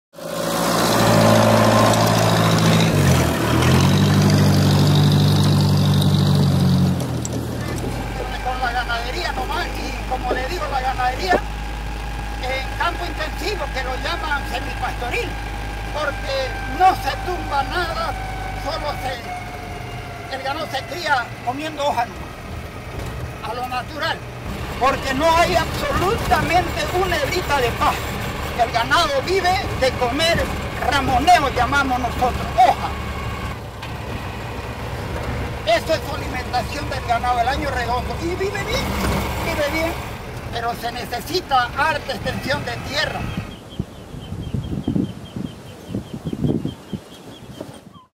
BOL381- Don Picky talks about himself
Don Luis Tipze speaks about his activities.